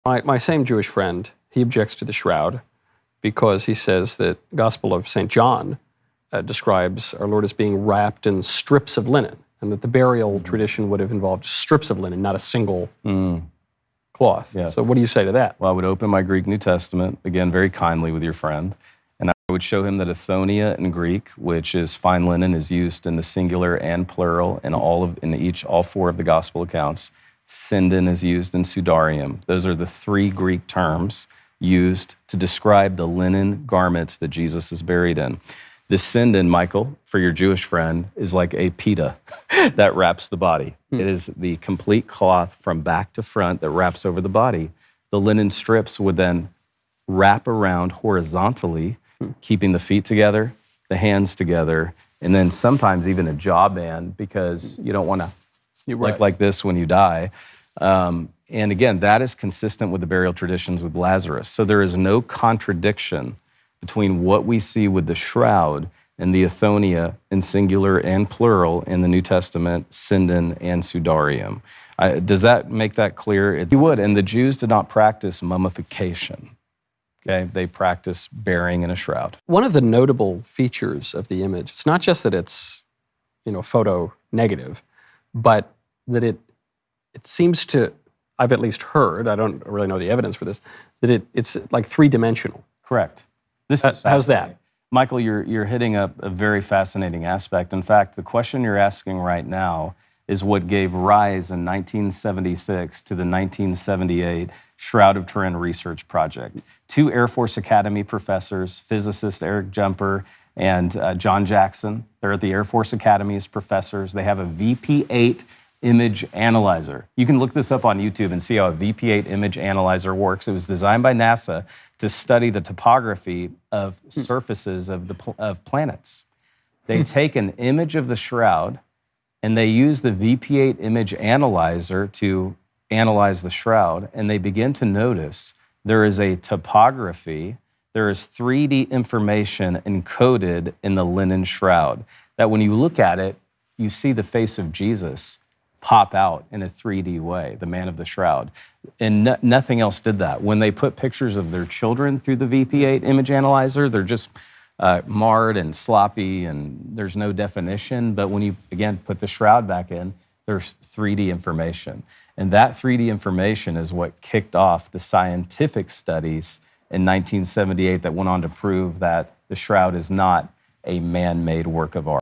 Shroud of Turin: Linen, Burial & 3D Secrets Revealed
We address common questions about the Shroud of Turin, examining the linen, burial traditions, and 3D image analysis. This video clarifies misconceptions and unveils the scientific studies conducted to prove the shroud's authenticity. Join our quest for answers!